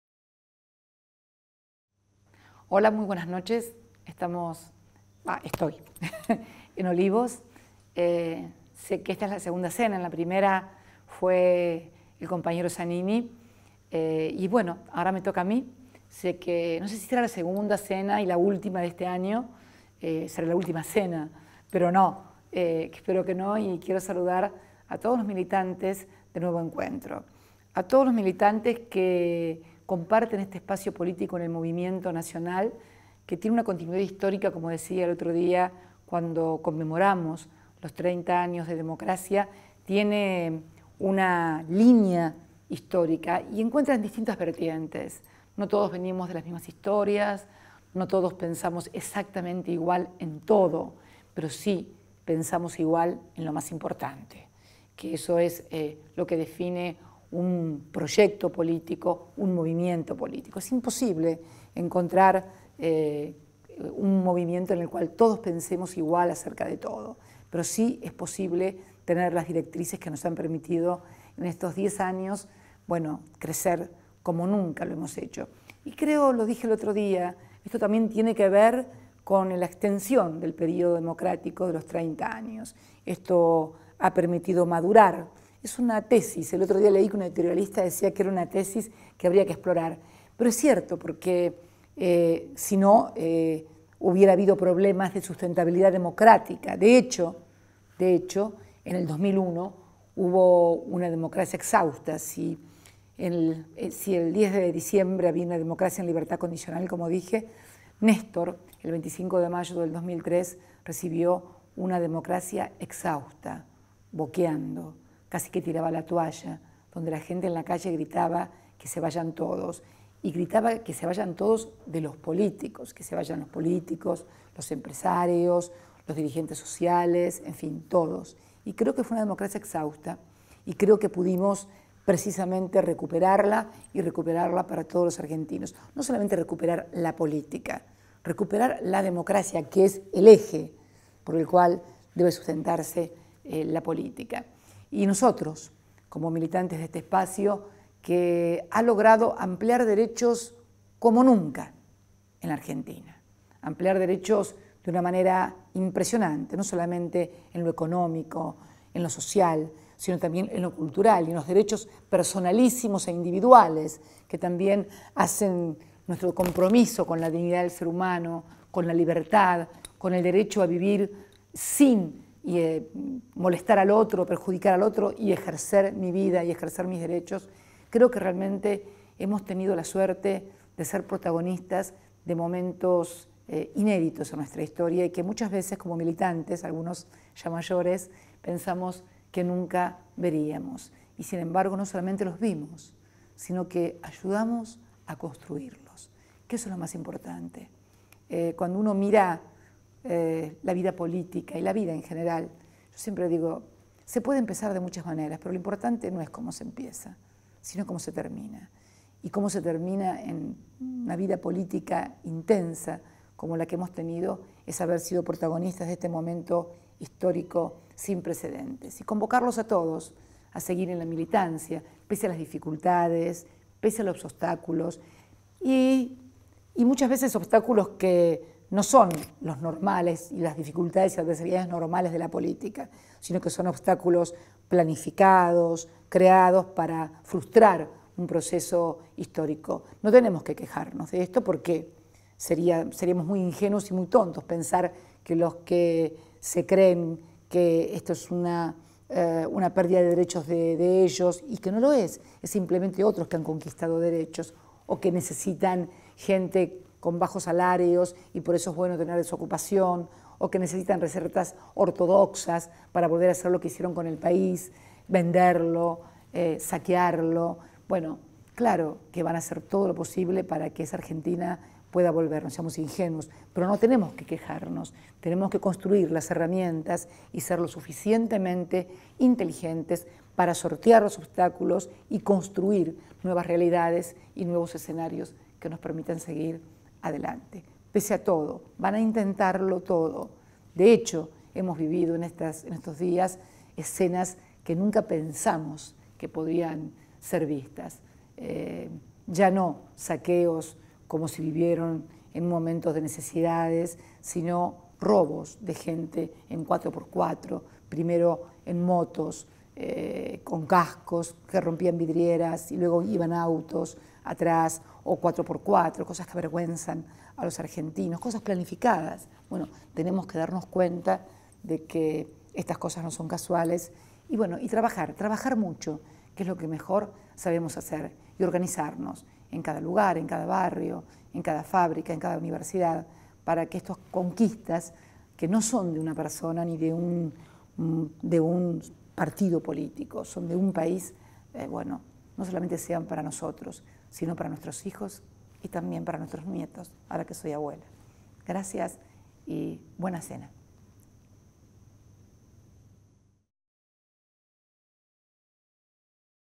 Saludo de Cristina Fernández de Kirchner a la militancia / Fin de año Nuevo Encuentro CABA
Emotivo saludo de nuestra presidenta Cristina Fernández de Kirchner a la militancia de Nuevo Encuentro en ocasión de la cena de fin de año de esa fuerza política en la Ciudad de Buenos Aires.